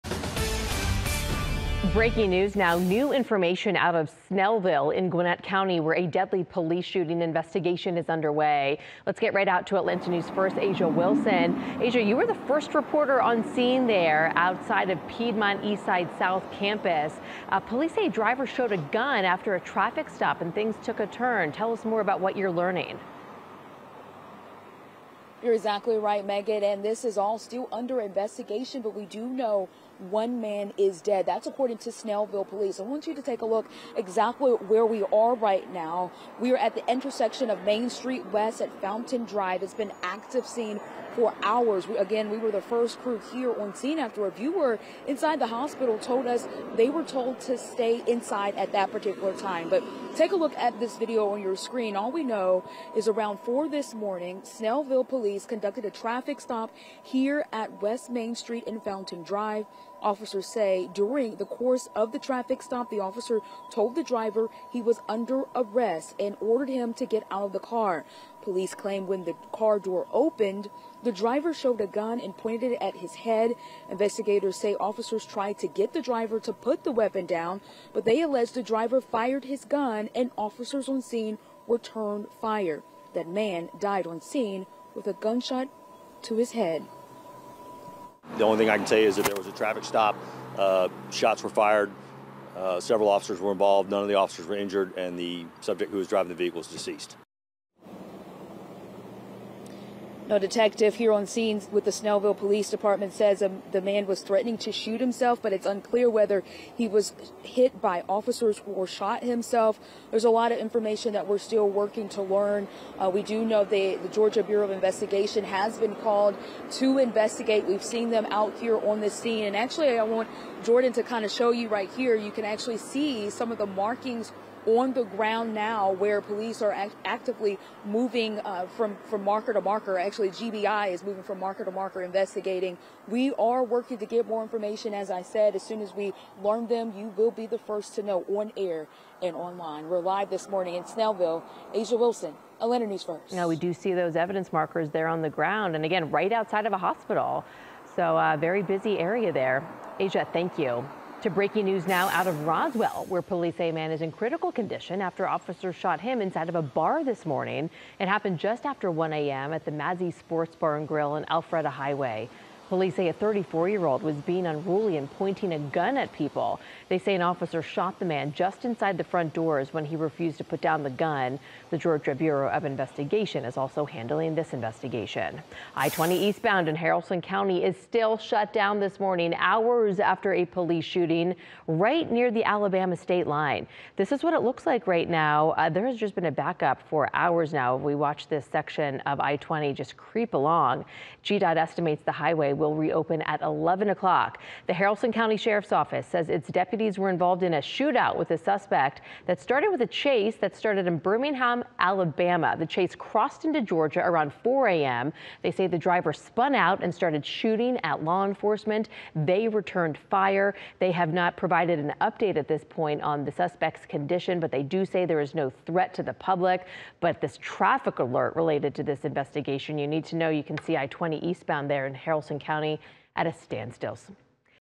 1 dead, 1 critically injured after several shootings involving officers in metro Atlanta
One shooting was in Snellville, one was in Roswell and the other was in Haralson County.